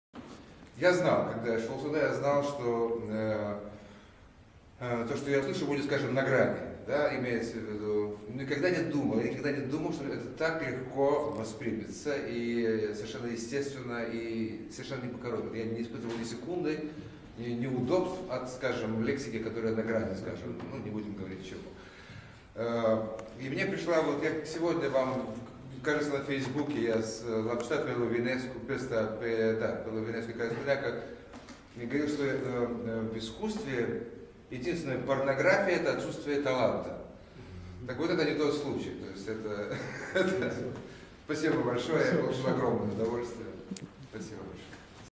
Corneliu Busuioc despre Vladimir Lorcenkov
Corneliu Busuioc despre Vladimir Lorcenkov la cenaclul Republica, Biblioteca Națională a Republicii Moldova. 7 februarie 2014